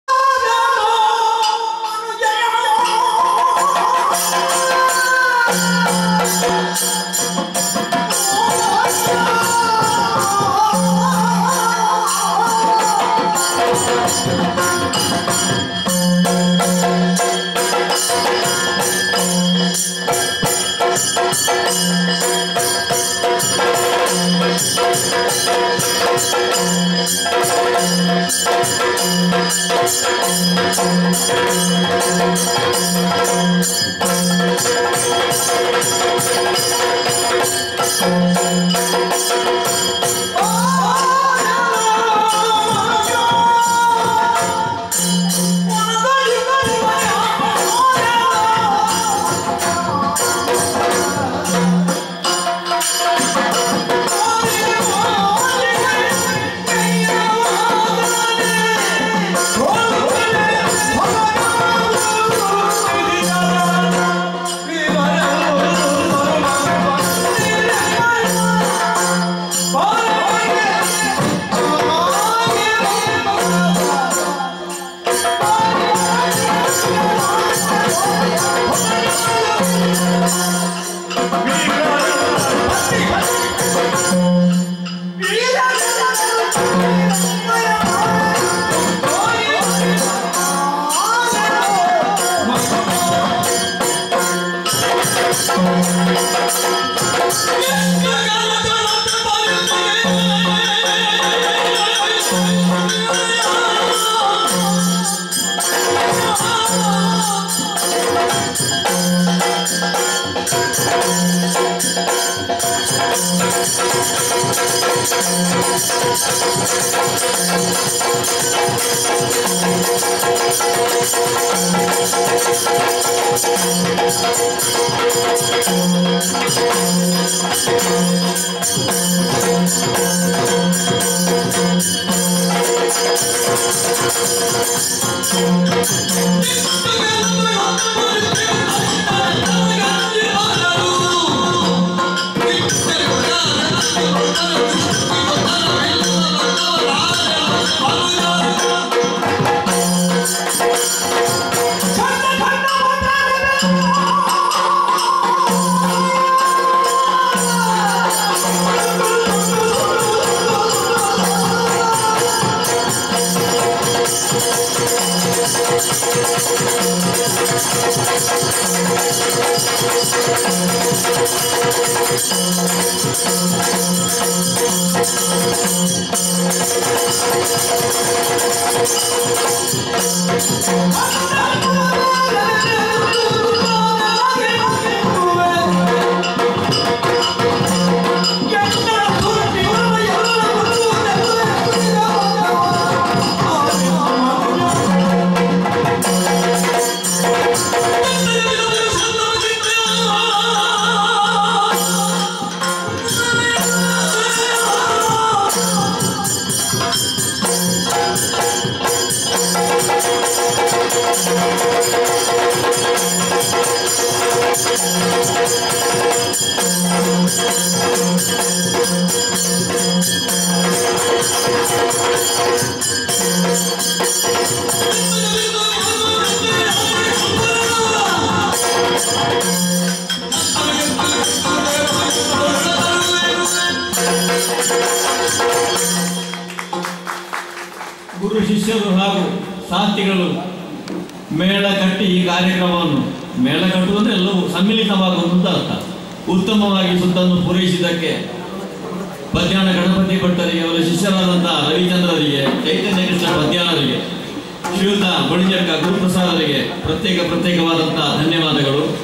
Yakshagana -- Yakshaganarchane - 22 - Eru padya....Padyana - Kannadikatte
Bhagavatharu Padyana Ganapathi Bhat & Ravichandra Kannadikatte -
Chende Guruprasad bhat Bolinjadka -
Maddale Chaithanya krishna Padyana -
Niroopane Subraya holla Kasaragod -
ಸಿರಿಬಾಗಿಲು ರಾಮಕೃಷ್ಣ ಮಯ್ಯರ ಸುವರ್ಣ ಗಾನಾರ್ಚನೆ
Held at Kuthyala Shri Gopalakrishna temple Kudlu ,on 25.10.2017